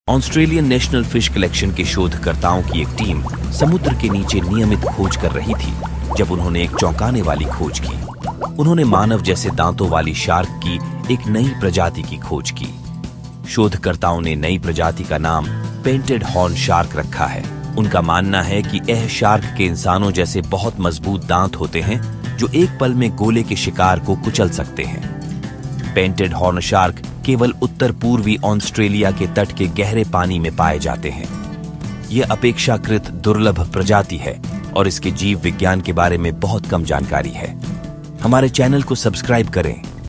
The painted hornshark (Heterodontus marshallae) is a recently described species of bullhead shark from northwestern Australia, described in 2023. It was previously considered conspecific with the Zebra bullhead The painted hornshark is a small shark, growing to about 60 centimeters (24 inches) in length. It has a brown or gray body with 22 dark brown bars and saddles. There is a dark brown blotch on the snout tip and a semicircular dark bar across the snout. The anal fin is well separated from the caudal fin.